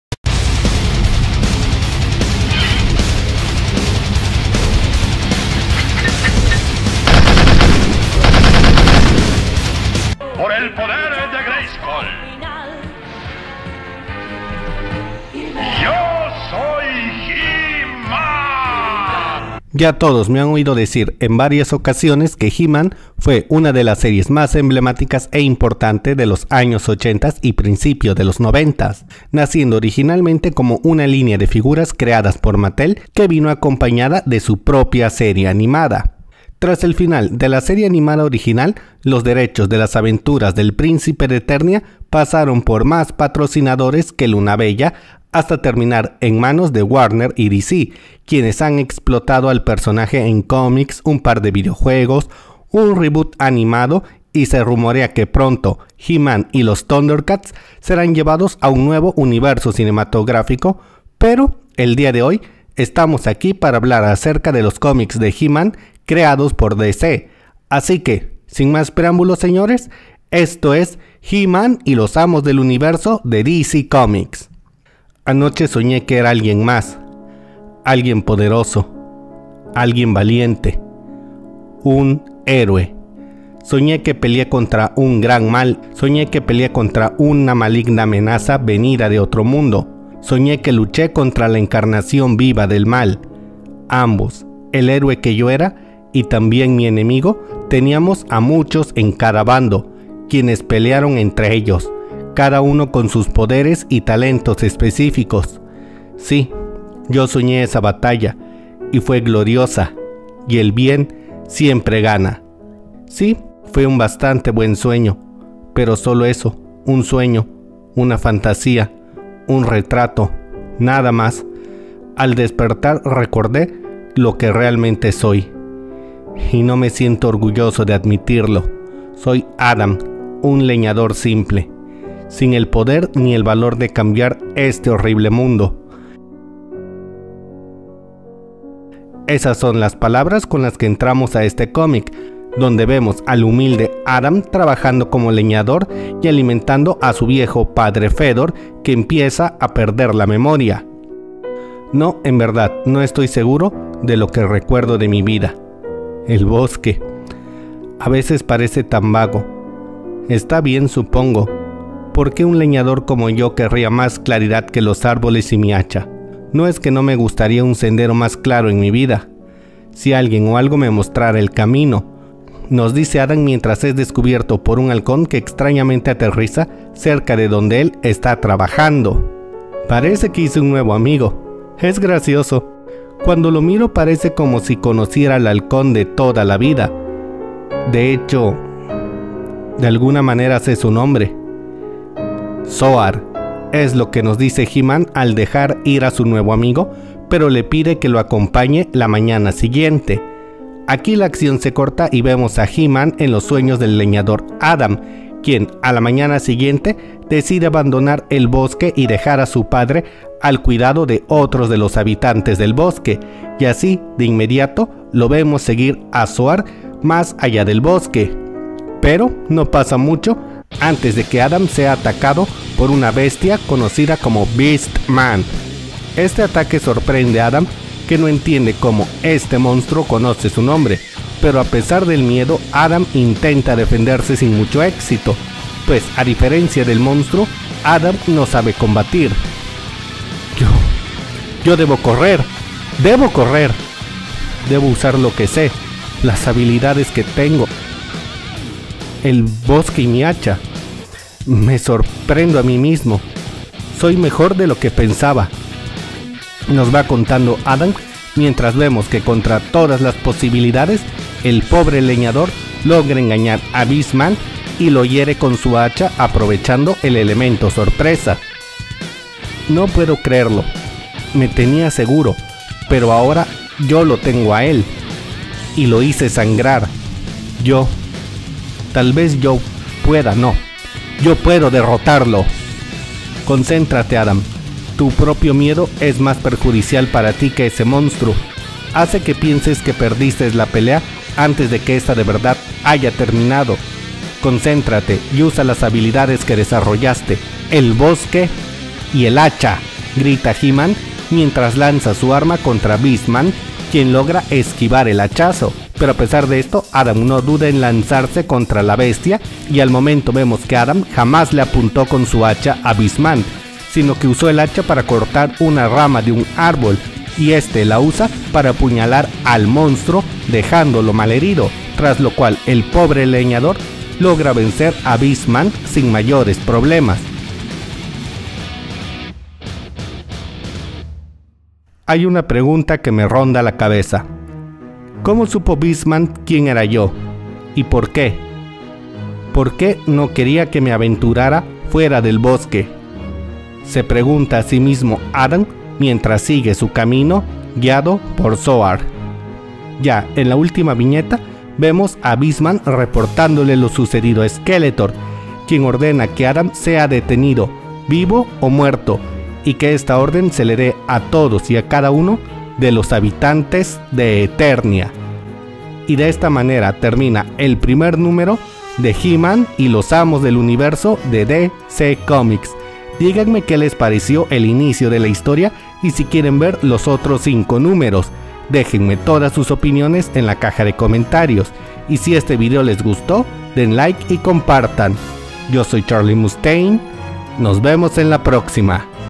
¡¡¡SKELETOR CONQUISTA ETERNIA!!!
Empecemos con una aventura de He-Man creada por DC Comics.